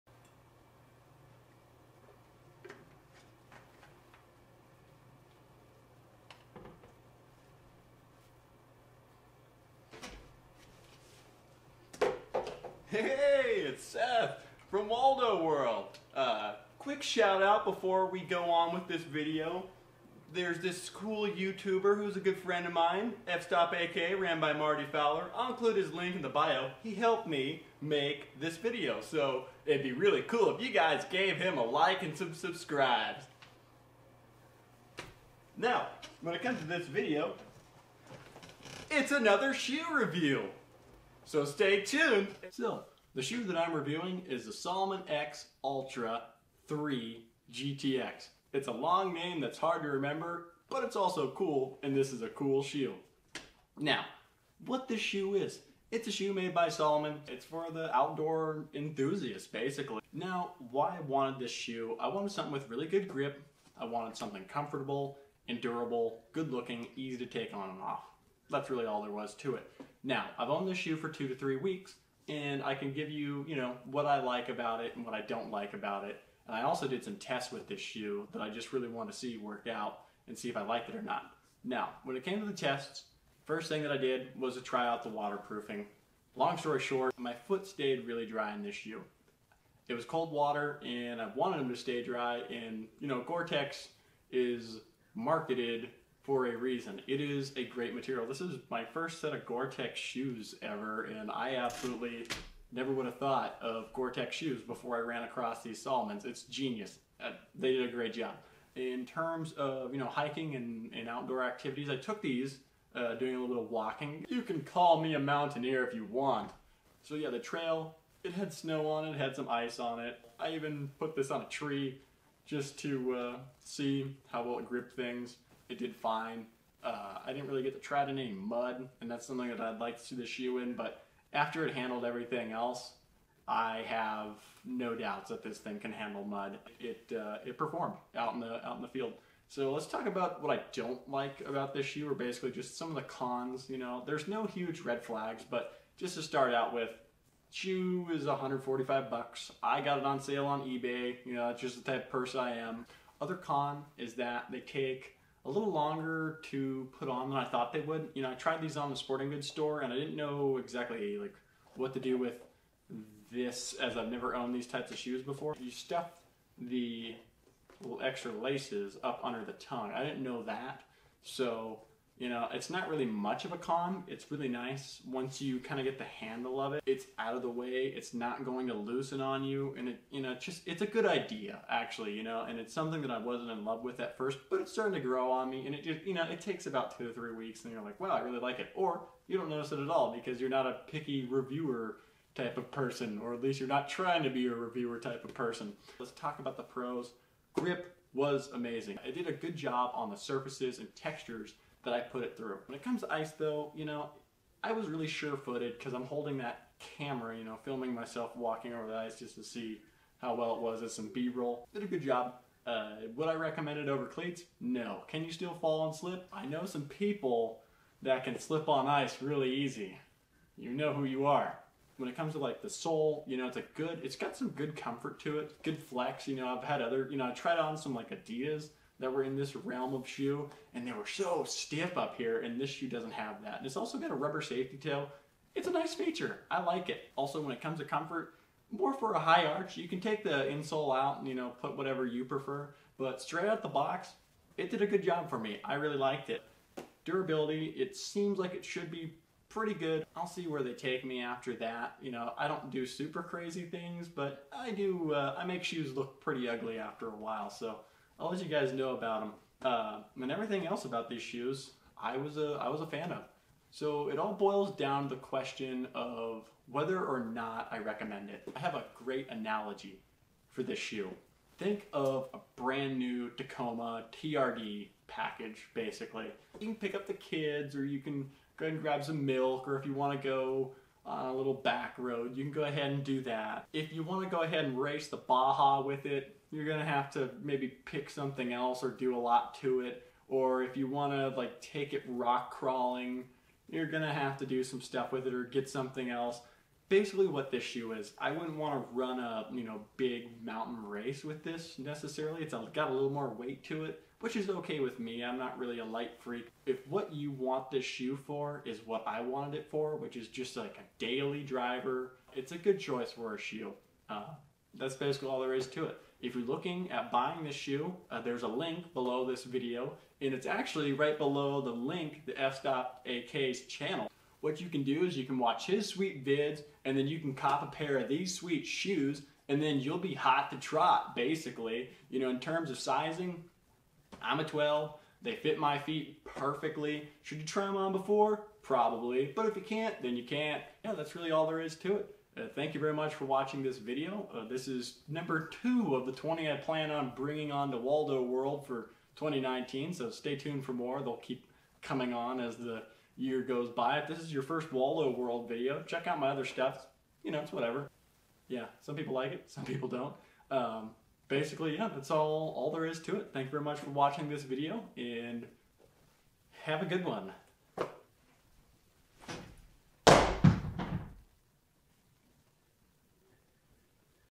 Salomon X Ultra 3 GTX - Shoe Review
If you're looking for information regarding the Salomon X Ultra 3 GTX, you're in luck! In this video, I give you my opinion on the shoe along with whether or not I recommend it. One thing I do recommend, though, is F-StopAK!